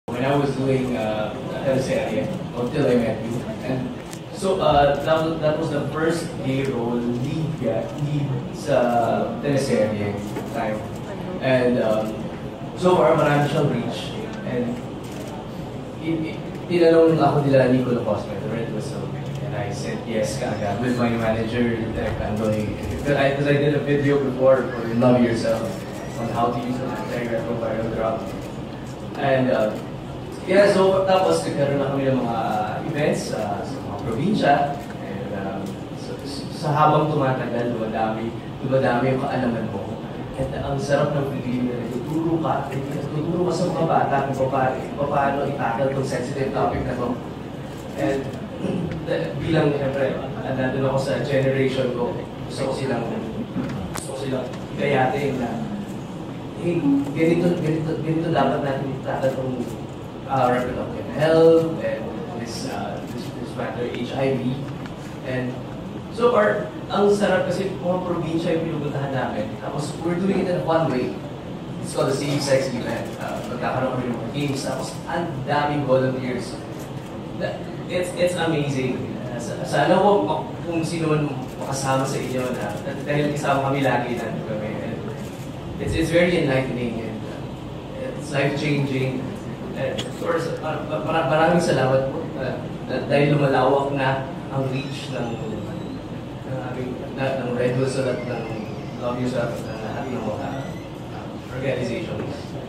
JC Santos finds fulfillment as Red Whistle ambassador
More videos on Rappler Follow Rappler for the latest news in the Philippines and across the globe. Actor JC Santos says his role in "Till I Met You" paved the way for his being an HIV/AIDS advocate. Full story